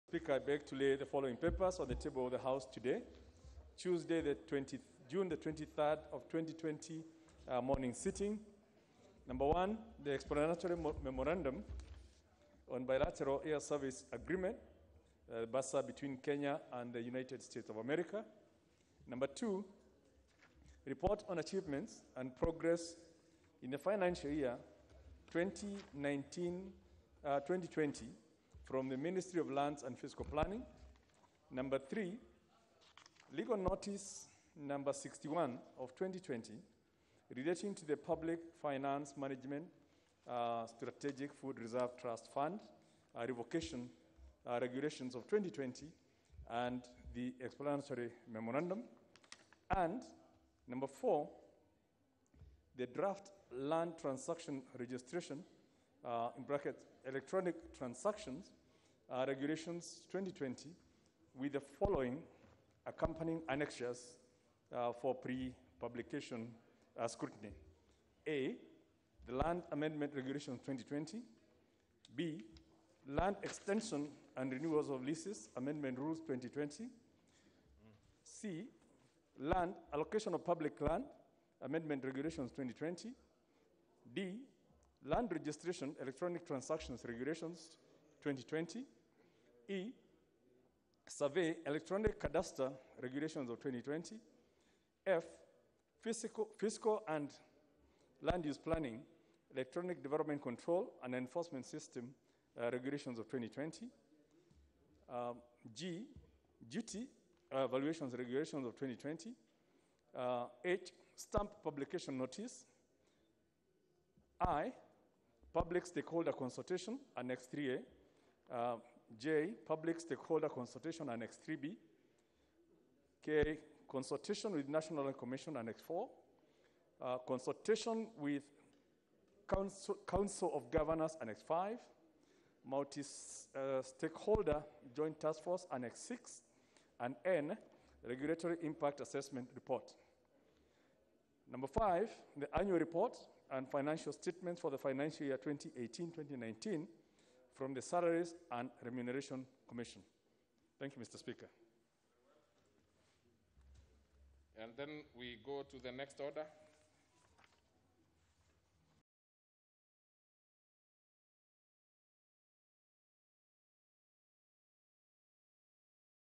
Kimunya starts off first House business as Majority leader
Amos Kimunya starts off first House business as Majority leader